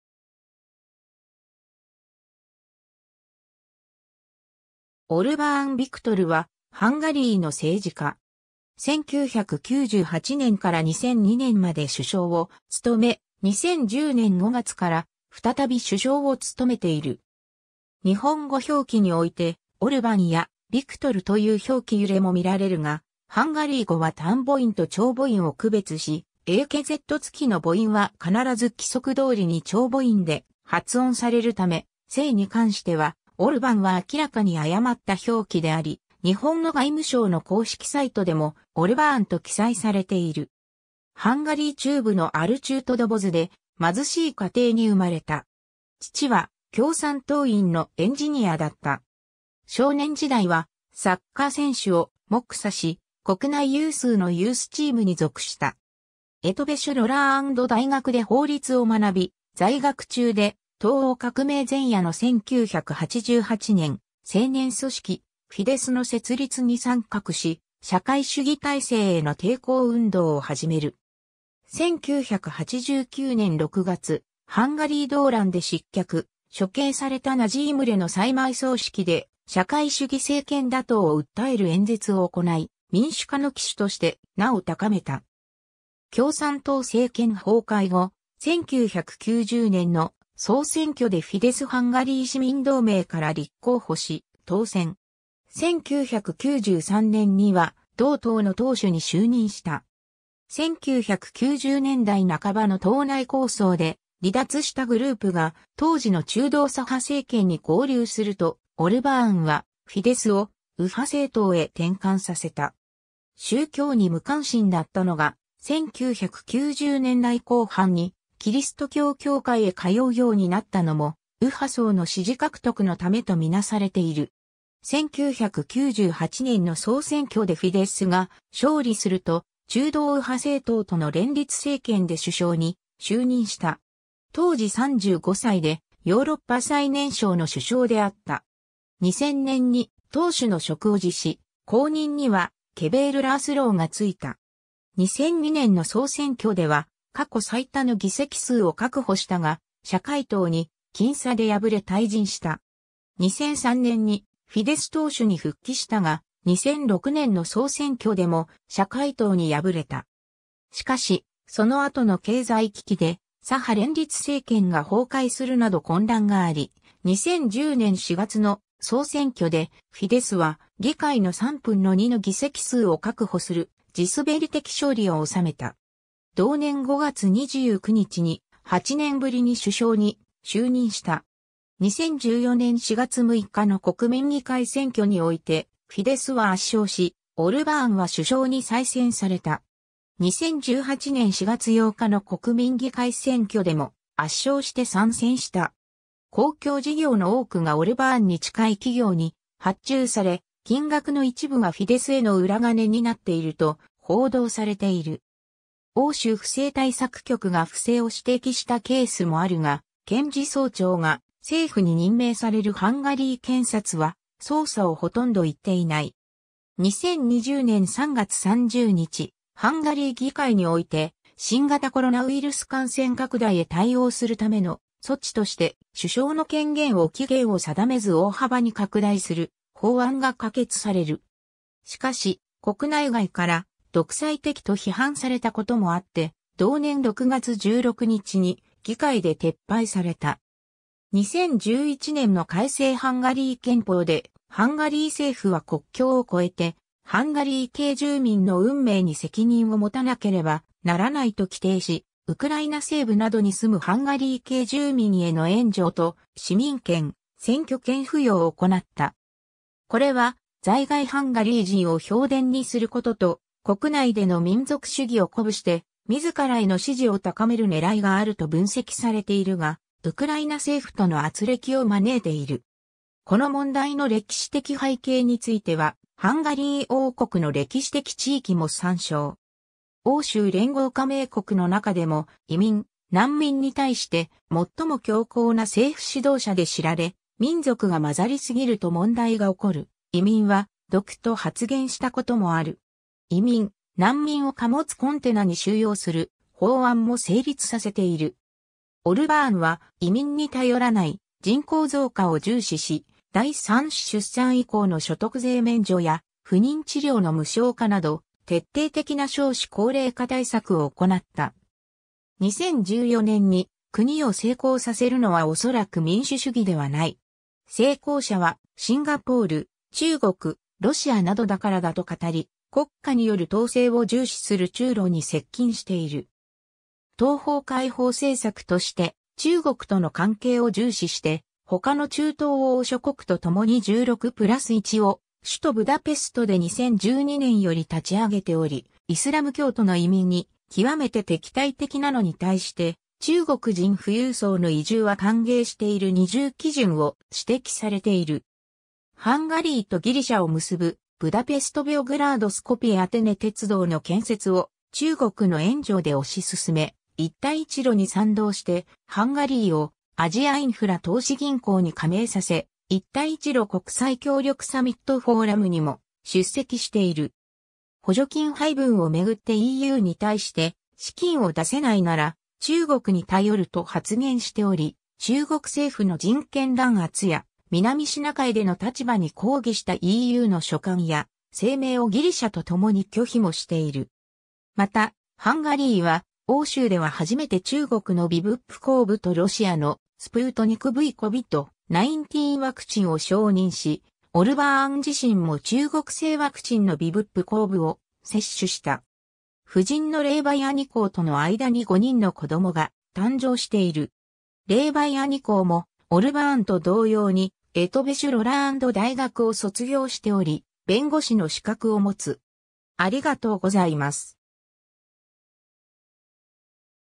オルバーン・ヴィクトル
オルバーン・ヴィクトル（ハンガリー語: Orbán Viktor、1963年5月31日 - ）は、ハンガリーの政治家。
1998年から2002年まで首相を務め、2010年5月から再び首相を務めている。
 日本語表記において「オルバン」や「ビクトル」という表記揺れも見られるが、ハンガリー語は（日本語と同様に）短母音と長母音を区別し、エーケゼット付きの母音は必ず規則通りに長母音で発音されるため、姓に関しては「オルバン」は明らかに誤った表記であり、日本の外務省の公式サイトでも「オルバーン」と記載されている。
 ハンガリー中部のアルチュートドボズ（首都ブダペスト近郊）で貧しい家庭に生まれた。
父は共産党員のエンジニアだった。
少年時代はサッカー選手を目指し、国内有数のユースチームに属した。
エトヴェシュ・ロラーンド大学（ブダペシュト大学）で法律を学び、在学中で東欧革命前夜の1988年、青年組織「フィデス」の設立に参画し、社会主義体制への抵抗運動を始める。
1989年6月、ハンガリー動乱で失脚、処刑されたナジ・イムレの再埋葬式で、社会主義政権打倒を訴える演説を行い、民主化の旗手として名を高めた。
 共産党政権崩壊後、1990年の総選挙でフィデス＝ハンガリー市民同盟から立候補し、当選。
1993年には同党の党首に就任した。
1990年代半ばの党内抗争で離脱したグループが当時の中道左派政権に合流すると、オルバーンはフィデスを右派政党へ転換させた。
宗教に無関心だったのが、1990年代後半にキリスト教教会へ通うようになったのも、右派層の支持獲得のためとみなされている。
 1998年の総選挙でフィデスが勝利すると、中道右派政党との連立政権で首相に就任した。
当時35歳で、ヨーロッパ最年少の首相であった。
2000年に党首の職を辞し、後任にはケヴェール・ラースローが就いた。
2002年の総選挙では過去最多の議席数を確保したが、社会党に僅差で敗れ退陣した。
 2003年にフィデス党首に復帰したが、2006年の総選挙でも社会党に敗れた。
しかし、その後の経済危機で左派連立政権が崩壊するなど混乱があり、2010年4月の総選挙でフィデスは議会の3分の2の議席数を確保する地滑り的勝利を収めた。
同年5月29日に、8年ぶりに首相に就任した。
 2014年4月6日の国民議会選挙において、フィデスは圧勝し、オルバーンは首相に再選された。
2018年4月8日の国民議会選挙でも圧勝して3選した。
 公共事業の多くがオルバーンに近い企業に発注され、金額の一部がフィデスへの裏金になっていると報道されている。
欧州不正対策局（OLAF）が不正を指摘したケースもあるが、検事総長が政府に任命されるハンガリー検察は捜査をほとんど行っていない。
 2020年3月30日、ハンガリー議会において、新型コロナウイルス感染拡大へ対応するための措置として、首相の権限を期限を定めず大幅に拡大する法案が可決される。
しかし、国内外から「独裁的」と批判されたこともあって同年6月16日に議会で撤廃された。
 2011年の改正ハンガリー憲法で「ハンガリー政府は国境を越えてハンガリー系住民の運命に責任を持たなければならない」と規定し、ウクライナ西部などに住むハンガリー系住民への援助と市民権・選挙権付与を行った。
これは在外ハンガリー人を票田にすることと、国内での民族主義を鼓舞して自らへの支持を高める狙いがあると分析されているが、ウクライナ政府との軋轢を招いている。
この問題の歴史的背景については「ハンガリー王国の歴史的地域」も参照。
 欧州連合加盟国の中でも移民・難民に対して最も強硬な政府指導者で知られ、「民族が混ざりすぎると問題が起こる」「移民は毒」と発言したこともある。
移民・難民を貨物コンテナに収容する法案も成立させている。
 オルバーンは移民に頼らない人口増加を重視し、第三子出産以降の所得税免除や不妊治療の無償化など徹底的な少子高齢化対策を行った。
 2014年に「国を成功させるのはおそらく民主主義ではない。成功者はシンガポール、中国、ロシアなどだからだ」と語り、国家による統制を重視する中ロに接近している。
 「東方開放政策」として中国との関係を重視して他の中東欧諸国（後に非旧共産圏のギリシャも参加する）とともに16プラス1...